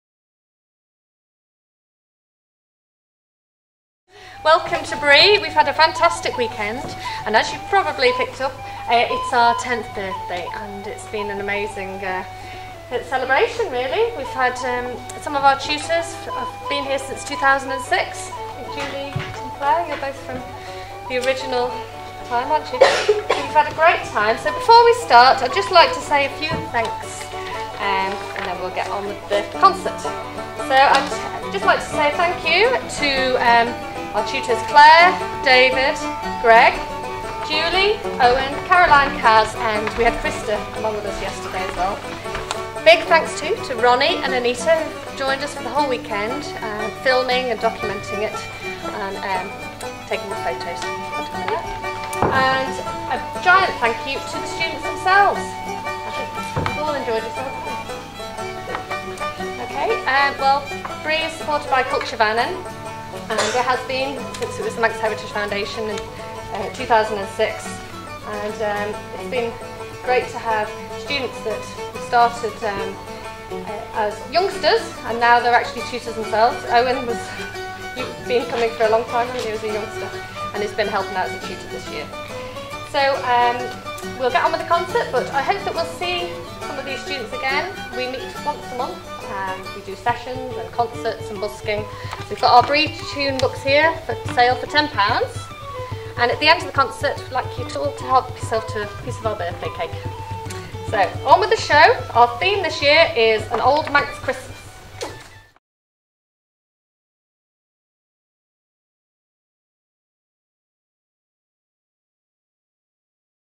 Introduction to the 2016 Bree concert by Dr Chloë Woolley
Dr Chloë Woolley introduces the 2016 concert at the conclusion of the Big Bree Workshop Weekend in the Youth Arts Centre, Douglas.
The full concert, as well as behind-the-scenes footage from the weekend, can be seen

This video was filmed by Ron Corkish. Thank you!